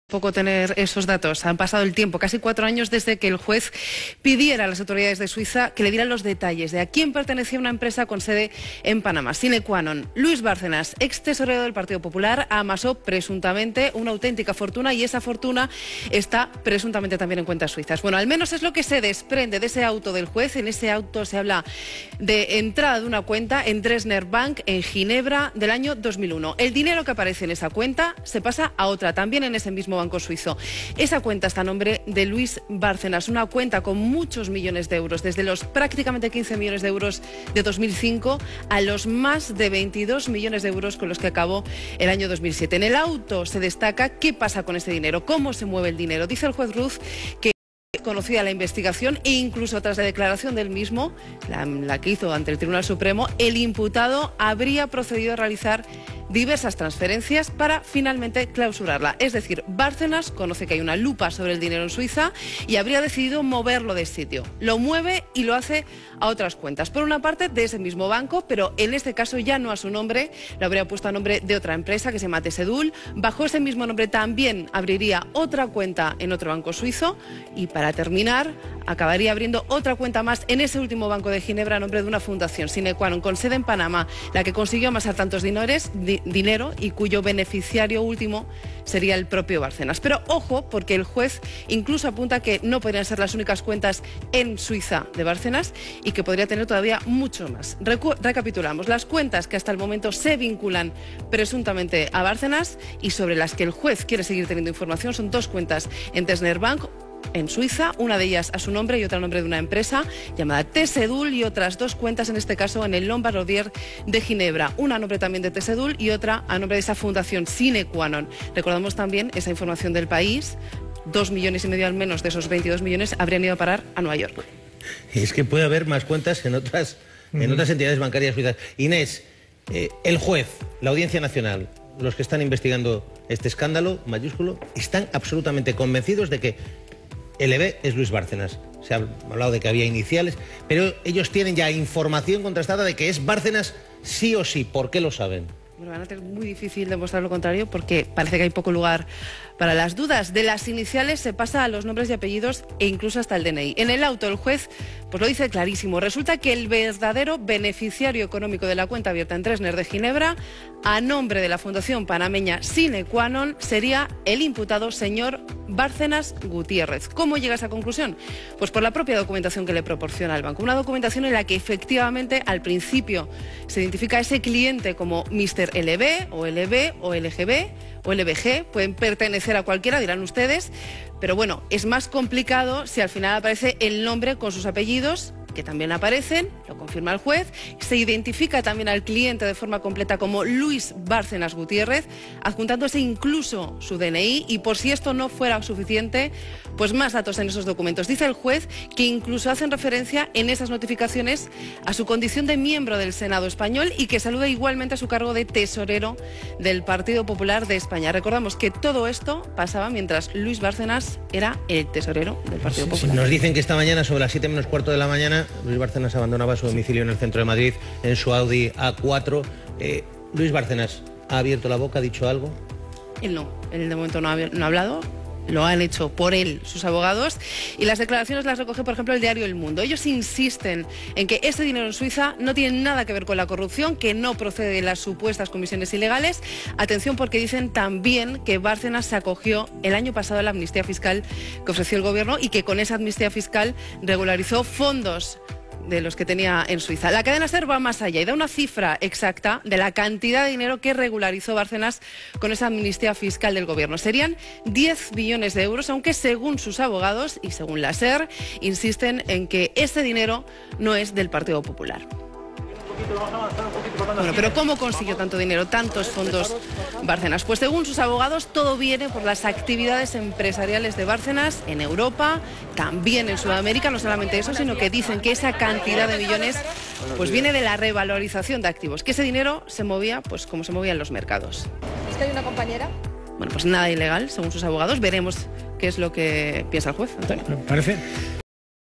El laberinto tributario construido en Suiza por Luis Bárcenas
Han pasado casi cuatro años desde que el juez pidiera a las autoridades helvéticas que le diera los detalles sobre el propietario de una empresa con sede en Panamá. Este era Luis Bárcenas, extesorero del Partido Popular que desarrolló un auténtico laberinto tributario en Suiza  con varias cuentas abiertas a su propio nombre y al de empresas y fundaciones.